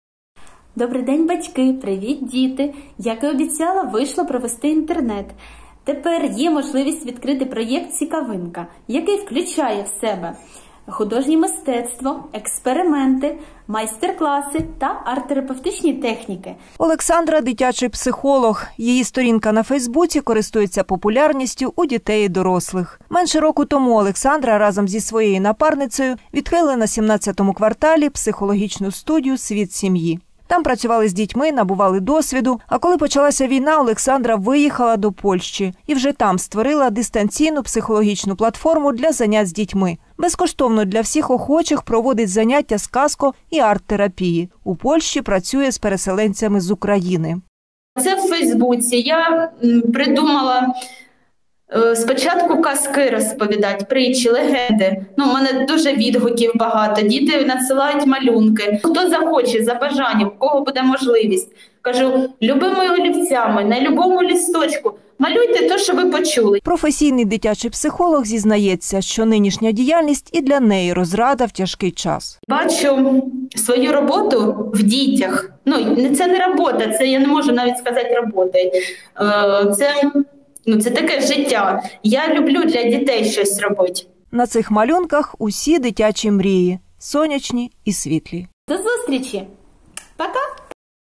Арт-терапія для дітей
Нині за кордоном, а думками зі своїми земляками. Тернівчанка Олександра Іщенко створила онлайн-проєкт, який набув популярності серед дітей та батьків. Про психологічну платформу, яка розраджує маленьких українців - далі.